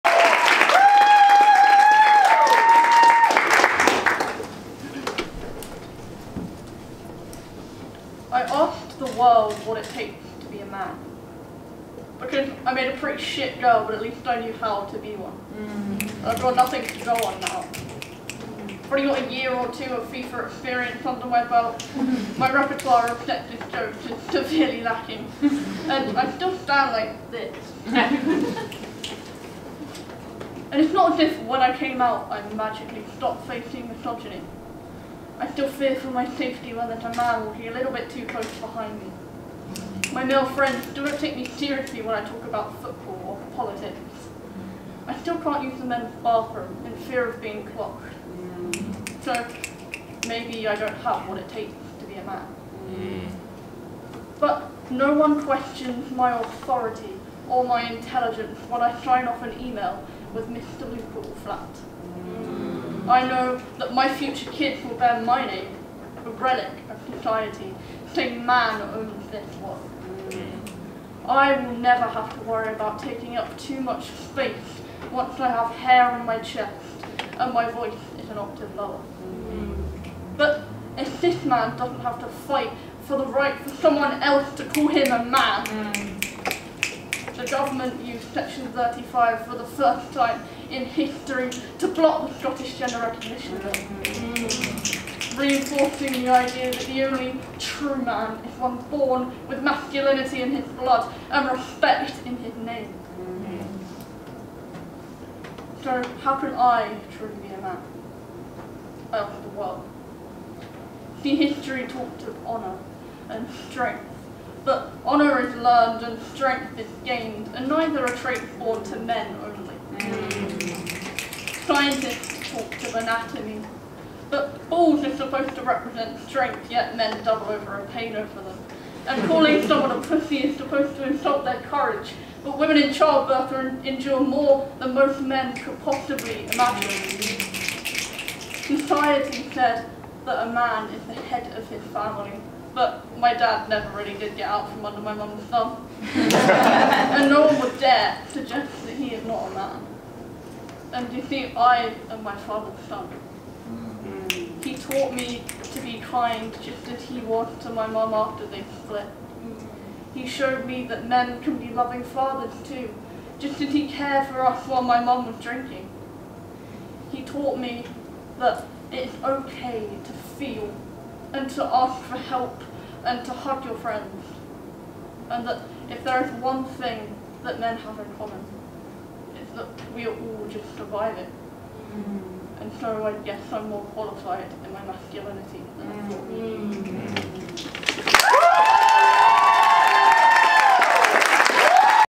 "I asked the world what it takes to be a man"- Luca Alflatt- Unislam 2023
This is a poem performed by Luca Alflatt in the first prelim of Unislam 2023.  In which Sheffield got to the final!  The footage was taken by Francis-Xavier Mukiibi who generously took the footage after we weren't sure we could film it ourselves and who also was a great support throughout the whole competition.

What does Luca have to have to say about the poem:
It's a poem to do with navigating toxic masculinity and gender dysphoria as a trans man. I found it really cool performing it at uni slam because it was such a welcoming environment and I felt like more people related to it than I anticipated.